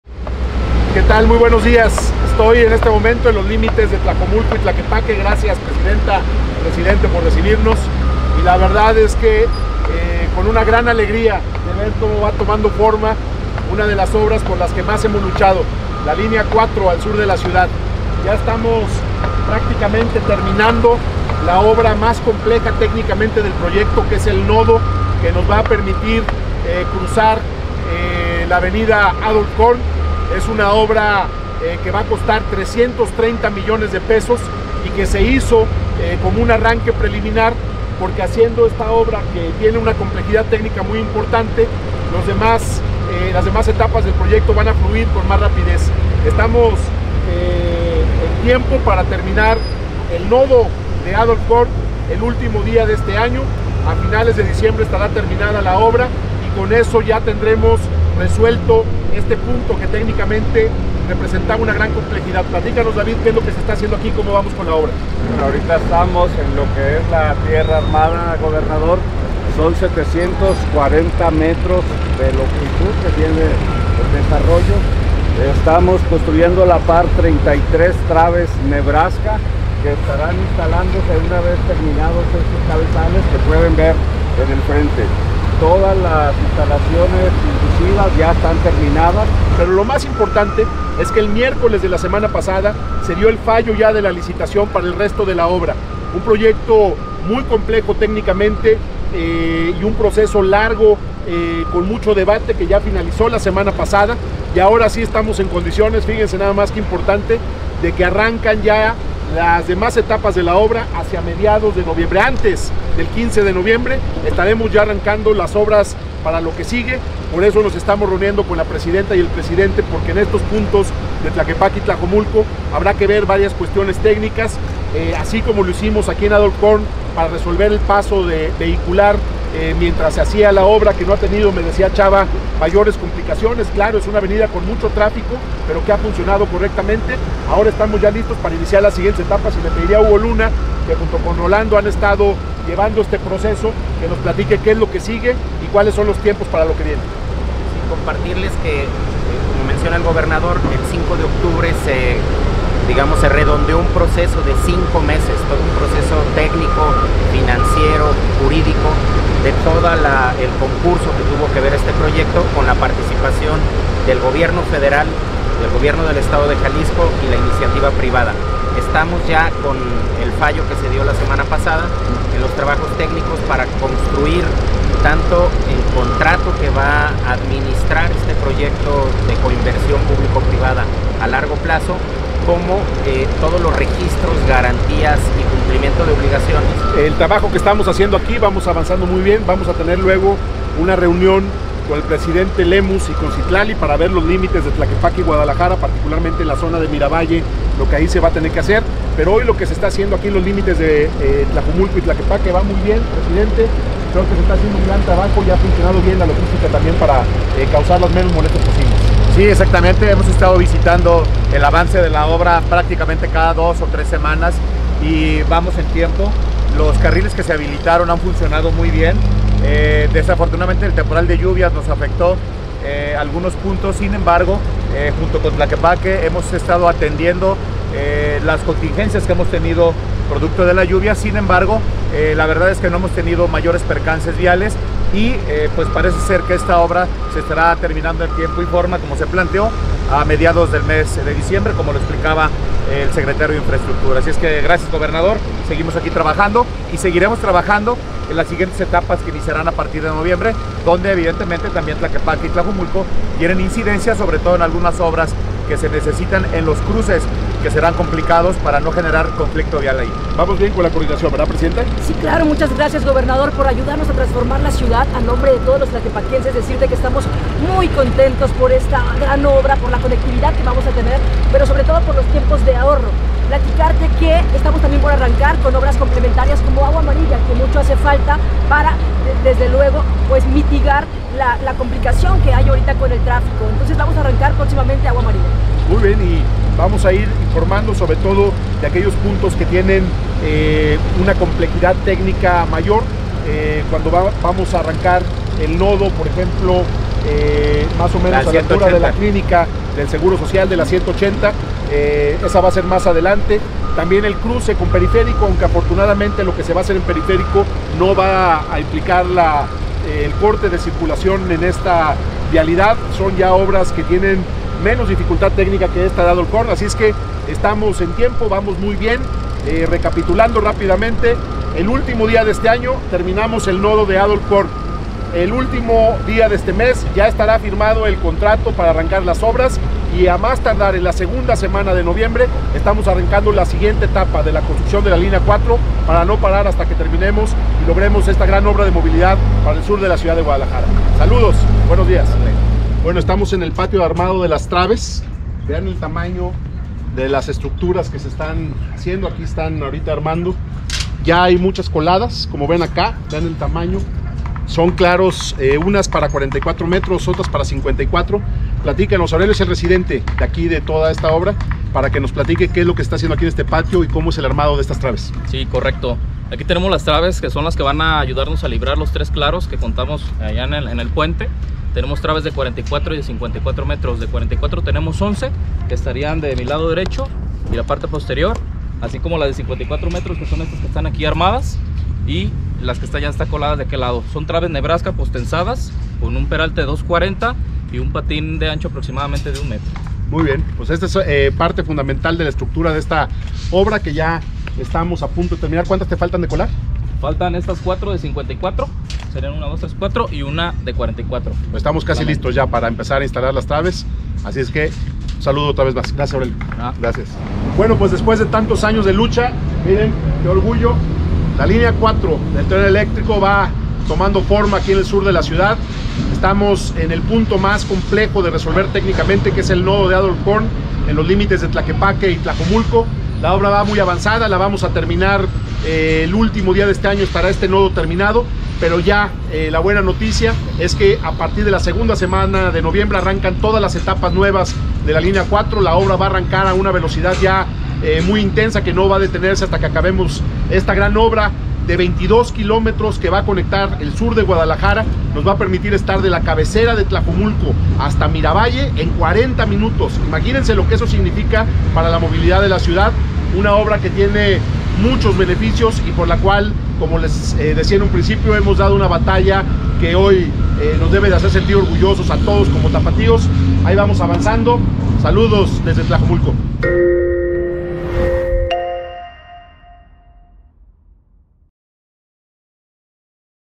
Línea 4 de Mi Tren | Nodo vial en Adolf Horn
Así va el nodo vial de Adolf Horn, que queda listo este mismo año. La Línea4 y el tren de Ferromex pasarán por abajo y los autos por arriba. Fueron 12 años de lucha, valen la pena 2 años de obras para darle rumbo a la movilidad del sur y toda la ciudad. Escucha qué sigue